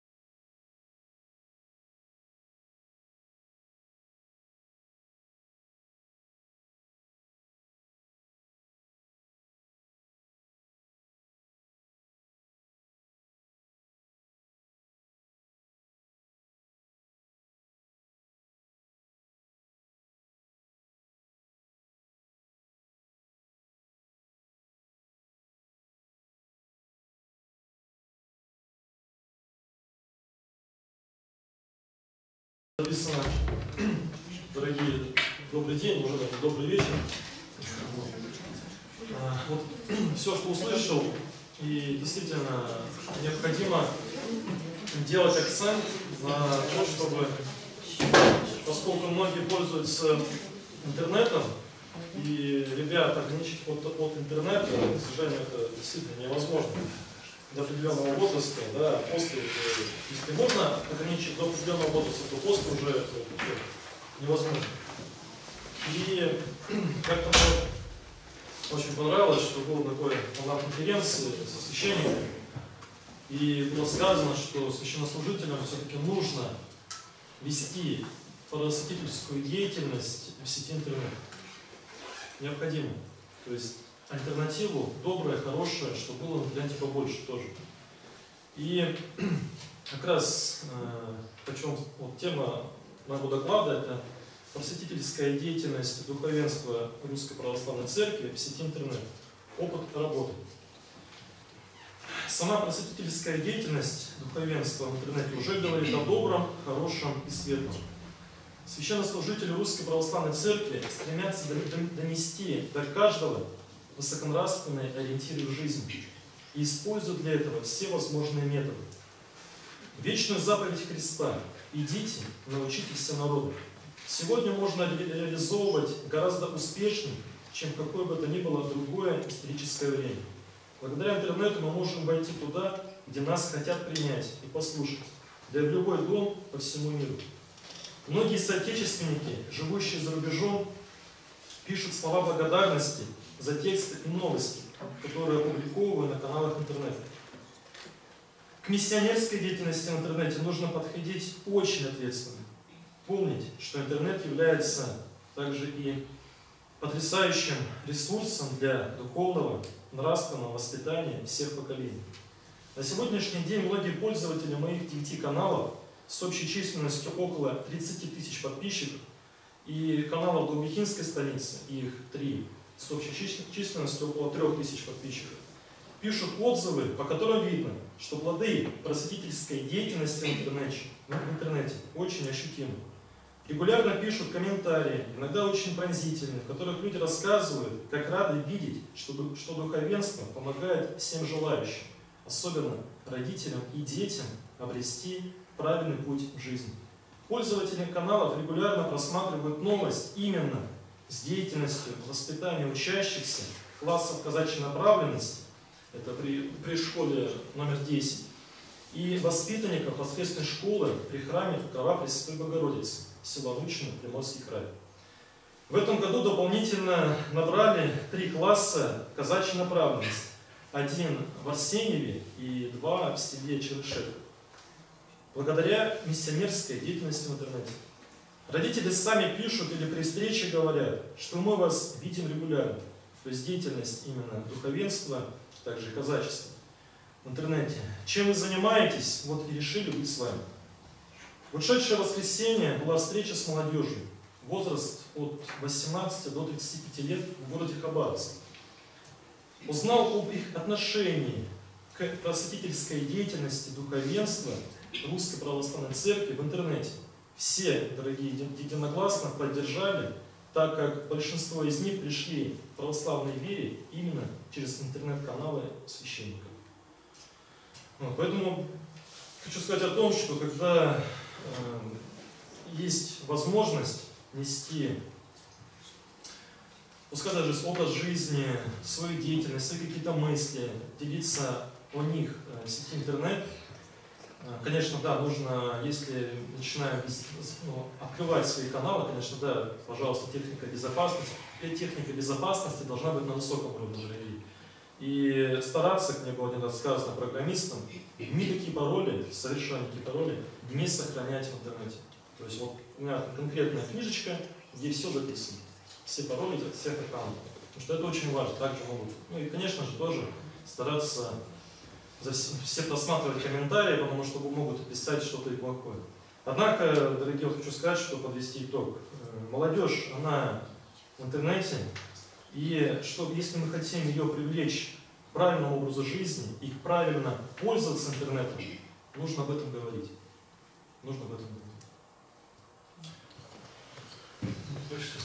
Иерей Димитрий Овчинников - Просветительская деятельность духовенства РПЦ в сети интернет
«Информационная безопасность в детской и молодёжной среде» – под таким названием 27 октября в Духовно-просветительском центре Арсеньевской епархии прошло заседание круглого стола. Мероприятие было подготовлено Отделом религиозного образования и катехизации. Оно состоялось в рамках епархиального этапа XXХI Международных Рождественских образовательных чтений «Глобальные вызовы современности и духовный выбор человека». В заседании приняли участие священнослужители и сотрудники ОРОиК, руководители школ, педагоги, родители, представители властных структур – всего 45 человек. Они обсудили актуальные вопросы духовной и психической безопасности ребёнка в условиях эскалации информационной войны.

Председатель собрания Преосвященнейший Гурий, епископ Арсеньевский и Дальнегорский, благословил участников на начало заседания. В своём докладе Владыка привёл слова Святейшего Патриарха всея Руси Кирилла о том, что сегодня приобретение духовного, политического и экономического суверенитета имеет для нас судьбоносное значение. Он отметил, что в течение более чем 400 лет наша страна была политическим и культурным придатком Запада, а сегодня у нас появилась возможность сбросить это иго. Против нас, против всего русского мира ополчился сам сатана. Что мы можем противопоставить нашим врагам? У Запада есть своя идеология, и нам тоже необходимо восстановить государственную идеологию как можно скорее, побеждать на образовательных и культурных полях сражений – это более важно, чем военные сражения. Нам нужно победить Запад прежде всего в самих себе, бороться за самобытность нашей культуры, наши ценности, обезопасить наших детей от видимого и невидимого зла.

Об острой актуальности поднятой проблемы и возможных путях её решения говорили почётные гости, которые по приглашению Владыки приняли участие в заседании круглого стола в онлайн-формате.  Так, из Москвы непосредственно перед заседанием российского парламента вышла с нами на связь Виктория Викторовна Николаева – Депутат Государственной Думы по Арсеньевскому избирательному округу. Она обратилась к участникам с приветствием и рассказала о том, именно в этот день, 27 октября, депутаты готовятся принять в первом чтении два очень важных закона: будут внесены изменения в Закон об информации, информационных технологиях и защите информации в части запрета пропаганды нетрадиционных сексуальных отношений, а также изменения в Кодекс РФ об административных правонарушениях – об административной  ответственности за их пропаганду.

Ольга Сергеевна Ивченко, и. о. директора Департамента внутренней политики Приморского края, начальник отдела по взаимодействию с институтами гражданского общества, рассказала присутствующим о роли некоммерческих объединений в патриотическом воспитании  детей в Приморье. Один из примеров – совместная деятельность Арсеньевской епархии и АНО «Восток».

Что делать в ситуации продолжающейся информационной опасности? С таким обращением к родителям и практическими советами выступила Ирина Яковлевна Медведева – директор Общественного института демографической безопасности, православный детский психолог, писатель, публицист, общественный деятель.

В обсуждении проблемы информационной безопасности приняли участие Ольга Петровна Янковая – специалист Управления образования АГО; Максим Александрович Карнажицкий – специалист зала электронных ресурсов МБУК ЦБС АГО. От Арсеньевской епархии в заседании приняли участие иерей Димитрий Овчинников – духовник Даубихинского казачьего общества, преподаватель Основ Православия в классах казачьей направленности МОБУ СОШ №10; инокиня Мария (Воробьёва) – руководитель грантовых проектов, реализуемых епархией и АНО «Восток»; Бердникова Ирина Геннадьевна – методист ОРОиК. Они рассказали о миссионерской деятельности духовенства в сети интернет, о совместных мероприятиях епархии и УО г. Арсеньева для детей и педагогов, о политике Губернатора Приморского края в отношении православного образования.

Подводя итог заседания, Владыка Гурий ещё раз подчеркнул важность совместных усилий Церкви и общества в борьбе за души детей.